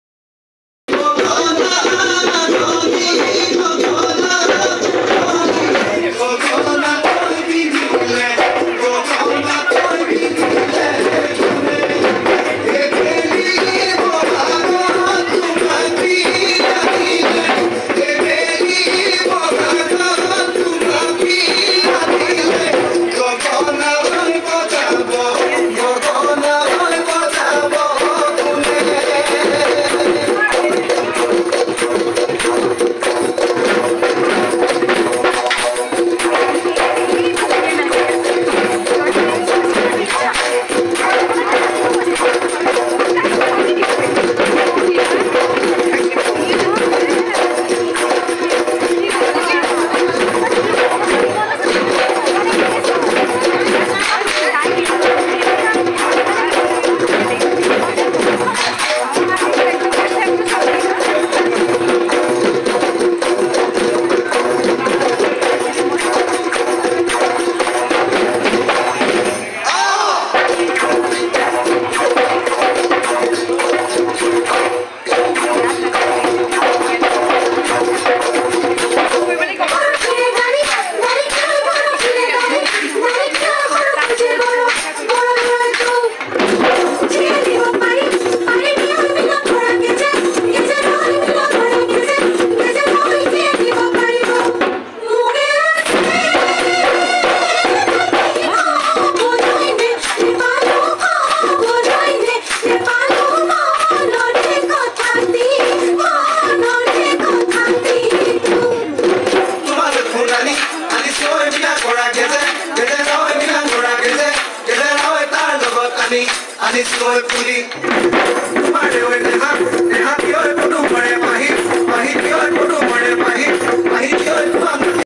Dhing College Cultural Program Today 2024 Bihu Dance At Dhing College//Shoriful's Vlogs//
keep Supporting